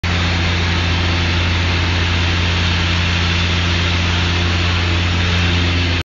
hydraulic ride on power trowel 2
our laser concrete screed is performancing on the show of bauma exhibition in shanghai 2016.